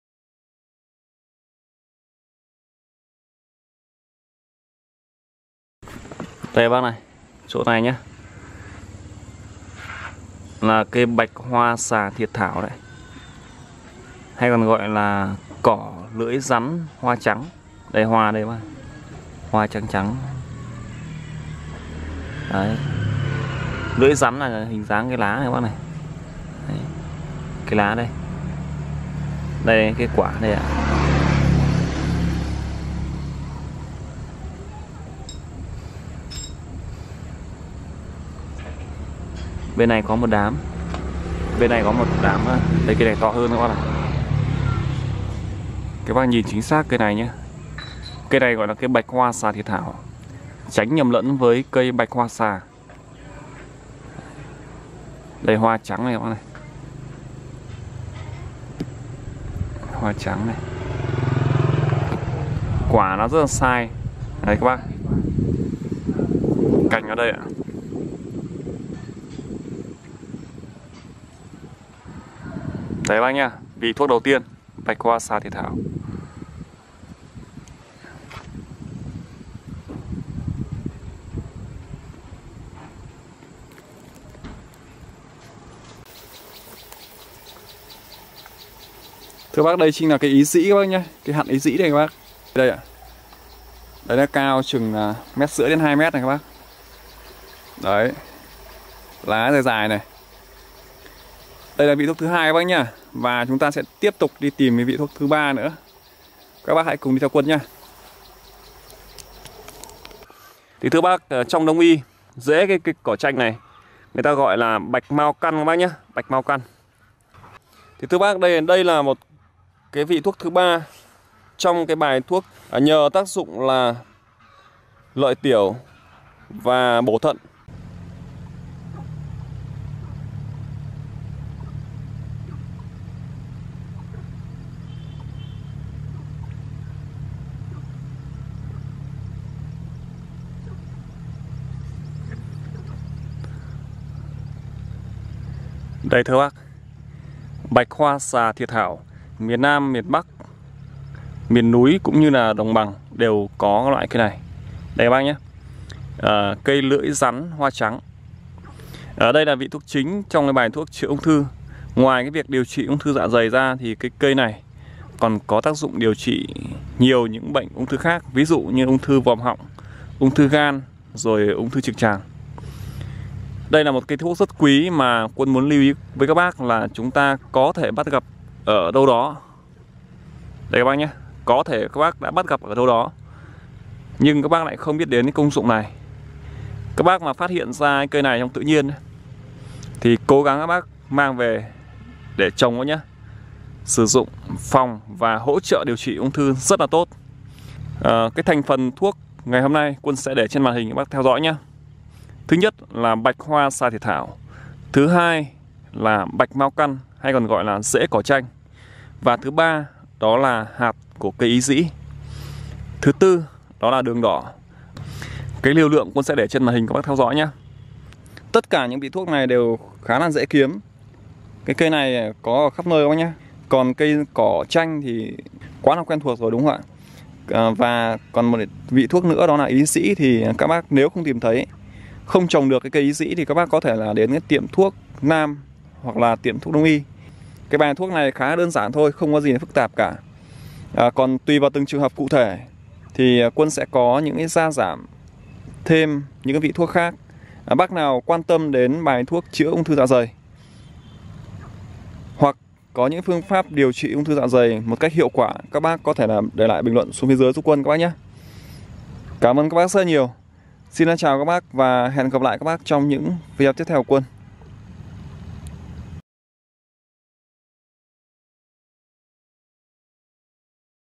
Khắc tinh U-Ng T-h.ư được cả thế giới săn lùng nhưng ở Việt Nam chỉ là cỏ dại | THAODUOC.NET
Bạch hoa xà thiệt thảo cây thuốc khắc tinh U- Ng T-h.ư được cả thế giới tôn vinh có khắp nơi ở nước ta nhưng nhiều người lại không biết.
Trong bài thuốc chữa U- Ng T-h.ư dạ dày này, bạch hoa xà thiệt thảo là vị chính kết hợp với các vị thuốc rất hay khác (Liều lượng trong video). Đây là toa căn bản để điều trị U- Ng T-h.ư dạ dày, có thể gia giảm thêm 1 vài vị trong những trường hợp cụ thể.
Lưu ý: Các bác tránh nhầm lẫn cây bạch hoa xà thiệt thảo với cây bạch hoa xà!